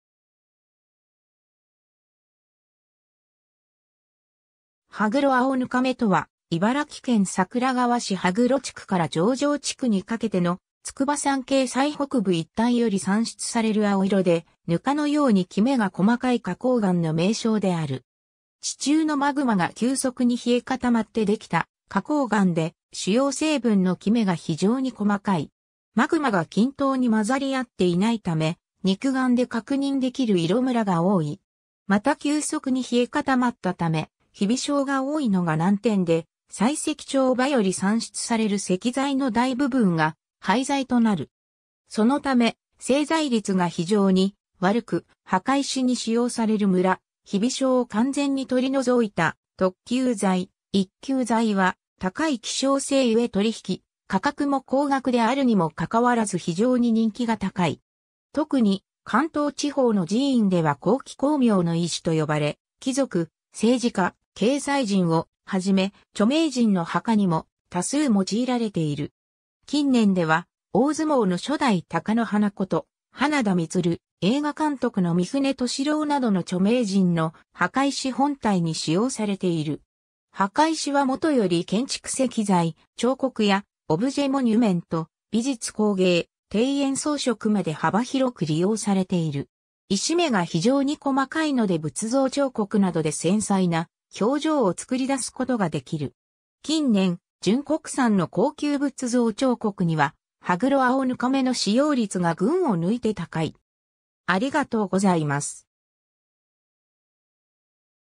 羽黒青糠目
羽黒青糠目（はぐろあおぬかめ）とは、茨城県桜川市（旧西茨城郡岩瀬町）羽黒地区から上城地区にかけての筑波山系最北部一帯より産出される青色で糠のようにキメが細かい花崗岩の名称である。
 地中のマグマが急速に冷え固まってできた花崗岩で、主要成分のキメが非常に細かい。
マグマが均等に混ざり合っていないため、肉眼で確認できる色ムラが多い。
また急速に冷え固まったため、ヒビ傷が多いのが難点で、採石丁場より産出される石材の大部分が廃材となる。
そのため製材率が非常に悪く、墓石に使用されるムラ、ヒビ傷を完全に取り除いた特級材、一級材は高い希少性ゆえ取引価格も高額であるにも関わらず非常に人気が高い。
 とくに関東地方の寺院では高貴光明の石と呼ばれ、貴族、政治家、経済人をはじめ、著名人の墓にも多数用いられている。
近年では大相撲の初代貴乃花こと花田満、映画監督の三船敏郎などの著名人の墓石本体に使用されている。
 墓石はもとより建築石材、彫刻やオブジェ・モニュメント、美術工芸、庭園装飾まで幅広く利用されている。
石目が非常に細かいので仏像彫刻などで繊細な表情を作りだすことが出来る。
近年、純国産の高級仏像彫刻には羽黒青糠目の使用率が群を抜いて高い。